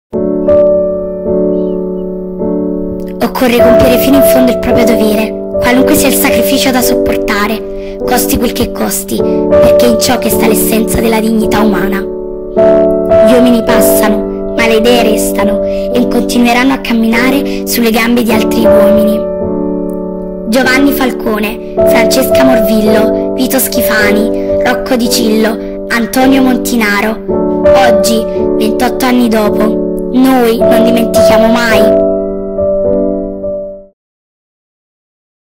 GIOVANNI FALCONE
Didattica a distanza
Filmato realizzato dalla nostra alunna An-Amy in collaborazione con l'Ambasciatrice eTwinning Cira Serio.
davinciresolve montaggiovideo Cira Serio Ambasciatrice eTwinning Italia
Grazie al sacrificio di uomini come Giovanni Falcone e Paolo Borsellino abbiamo imparato che vale sempre la pena di lottare per la Giustizia, per la Verità, per i più deboli e soprattutto abbiamo imparato che la paura non ci fermerà MAI!!! Grazie Giovanni! Non ti dimenticheremo MAI!!!
Gli uomini passano, le idee restano. Restano le loro tensioni morali e continueranno a camminare sulle gambe di altri uomini. Giovanni Falcone